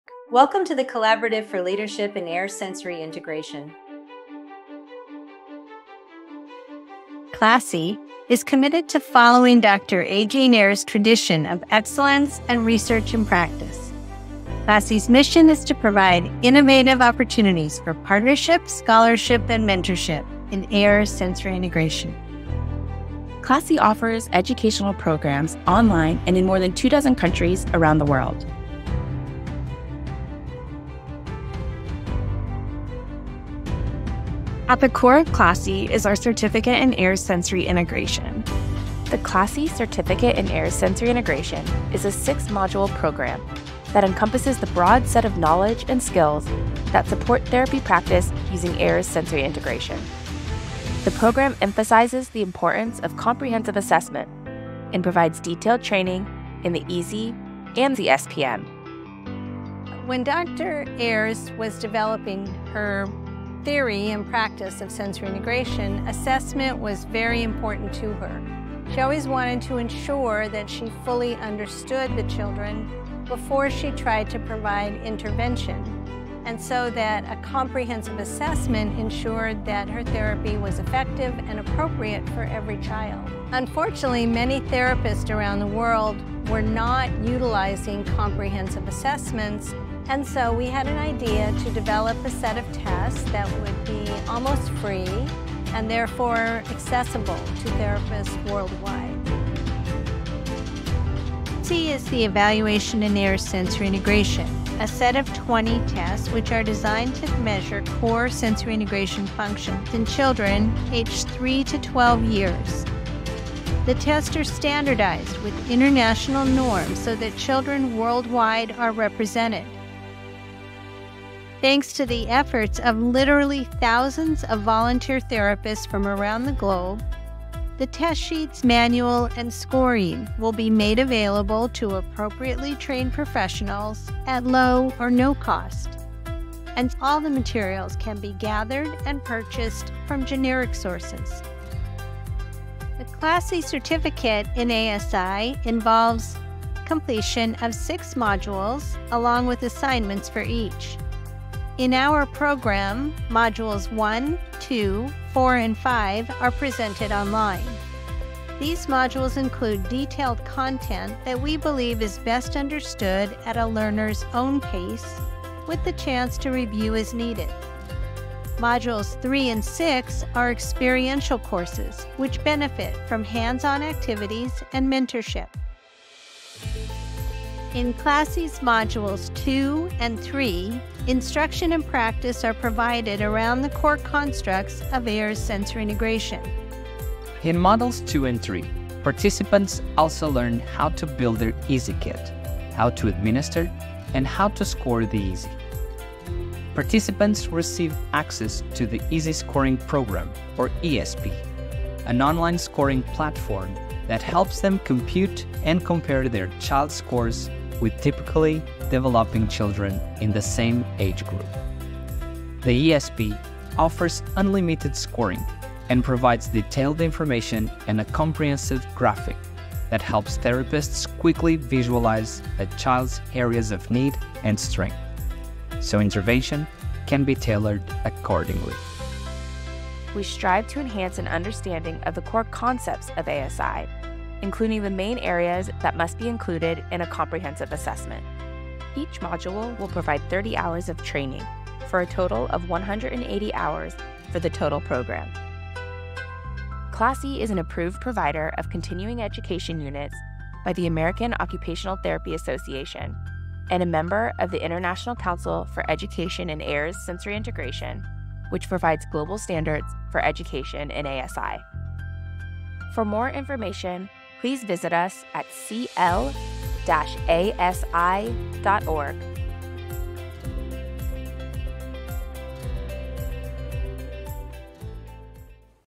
CLASI Certificate in Ayres Sensory Integration®
Welcome to the Collaborative for Leadership in Ayres Sensory Integration.

CLASI is committed to following Dr. A. Jean Ayres' tradition of excellence in research and practice.
CLASI's mission is to provide innovative opportunities for partnership, scholarship, and mentorship in Ayres Sensory Integration.
CLASI offers educational programs online and in more than two dozen countries around the world.

At the core of CLASI is our Certificate in Ayres Sensory Integration.

The CLASI Certificate in Ayres Sensory Integration is a six-module program that encompasses the broad set of knowledge and skills that support therapy practice using Ayres Sensory Integration.

The program emphasizes the importance of comprehensive assessment and provides detailed training in the EASI and the SPM.

When Dr. Ayres was developing her theory and practice of sensory integration, assessment was very important to her. She always wanted to ensure that she fully understood the children before she tried to provide intervention.

A comprehensive assessment ensured that her therapy was effective and appropriate for every child. Unfortunately, many therapists around the world were not utilizing comprehensive assessments, so we had an idea to develop a set of tests that would be almost free and therefore accessible to therapists worldwide.

The EASI is the Evaluation in Ayres Sensory Integration, a set of 20 tests designed to measure core sensory integration functions in children aged 3 to 12 years. The tests are standardized with international norms so that children worldwide are represented. Thanks to the efforts of literally thousands of volunteer therapists from around the globe, the test sheets, manual, and scoring will be made available to appropriately trained professionals at low or no cost. All the materials can be gathered and purchased from generic sources.

The CLASI Certificate in ASI involves completion of six modules along with assignments for each. In our program, Modules 1, 2, 4, and 5 are presented online. These modules include detailed content that we believe is best understood at a learner's own pace, with the chance to review as needed.

Modules 3 and 6 are experiential courses, which benefit from hands-on activities and mentorship.

In CLASI's Modules 2 and 3, instruction and practice are provided around the core constructs of Ayres Sensory Integration. In Modules 2 and 3, participants also learn how to build their EASI kit, how to administer, and how to score the EASI.

Participants receive access to the EASI Scoring Program, or ESP—an online scoring platform that helps them compute and compare their child scores with typically developing children in the same age group. The ESP offers unlimited scoring and provides detailed information and a comprehensive graphic that helps therapists quickly visualize a child's areas of need and strength, so intervention can be tailored accordingly.

We strive to enhance an understanding of the core concepts of ASI, including the main areas that must be included in a comprehensive assessment.

Each module will provide 30 hours of training for a total of 180 hours for the total program.

CLASI is an approved provider of continuing education units by the American Occupational Therapy Association and a member of the International Council for Education in Ayres Sensory Integration (ICEASI), which provides global standards for education in ASI.